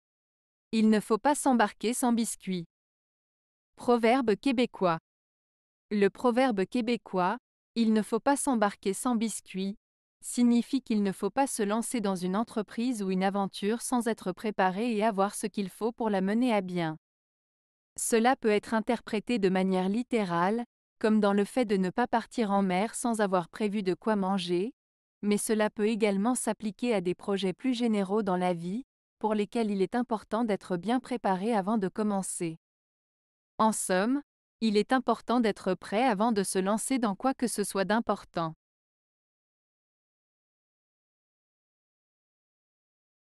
Il ne faut pas s'embarquer sans biscuit. - Proverbe Québécois
Le proverbe québécois "Il ne faut pas s'embarquer sans biscuit" signifie qu'il ne faut pas se lancer dans une entreprise ou une aventure sans être préparé et avoir ce qu'il faut pour la mener à bien. Cela peut être interprété de manière littérale, comme dans le fait de ne pas partir en mer sans avoir prévu de quoi manger, mais cela peut également s'appliquer à des projets plus généraux dans la vie, pour lesquels il est important d'être bien préparé avant de commencer. En somme, il est important d'être prêt avant de se lancer dans quoi que ce soit d'important.